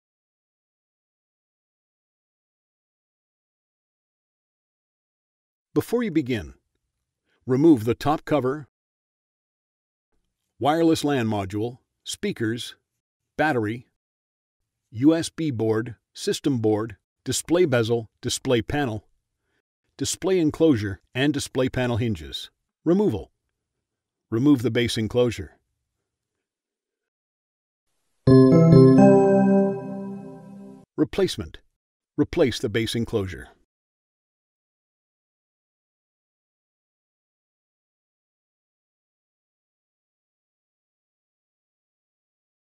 Replace the Base Enclosure | HP Chromebook 11 G7 EE, 11 G6 EE
Learn how to replace the base enclosure for HP Chromebook 11 G7 EE, 11 G6 EE.